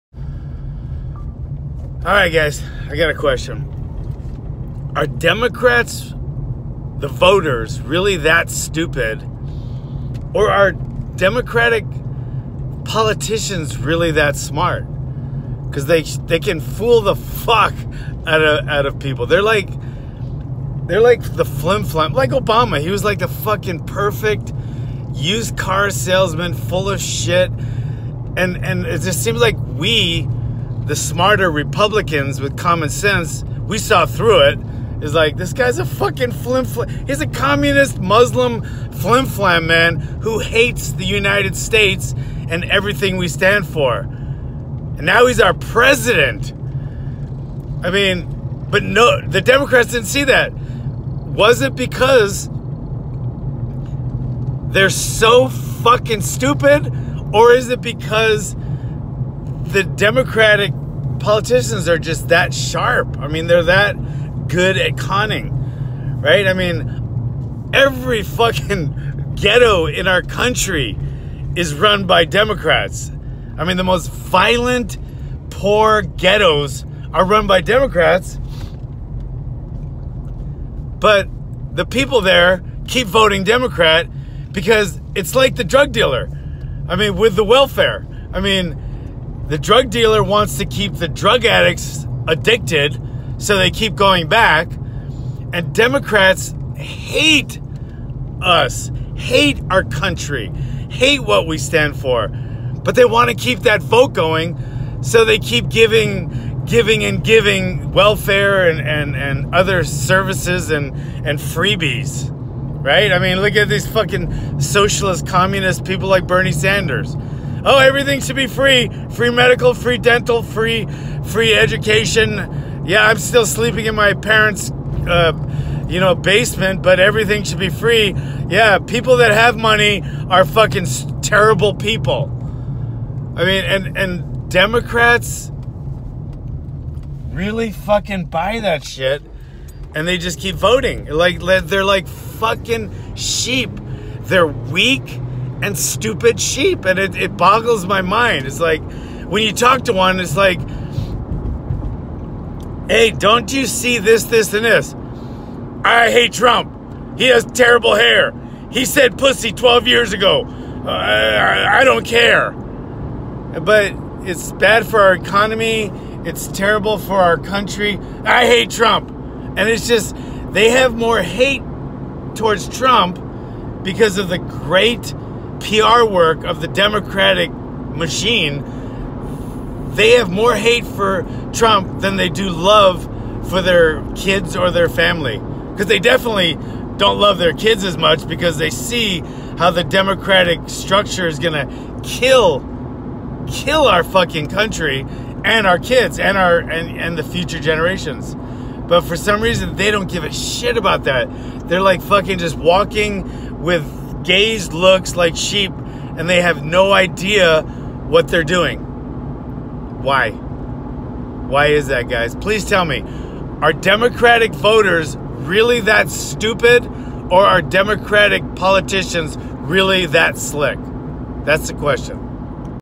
Our Democratic politicians really that smart?
Democratic politicians are very very smart, or else Democratic voters are just very very stupid, there’s no in between.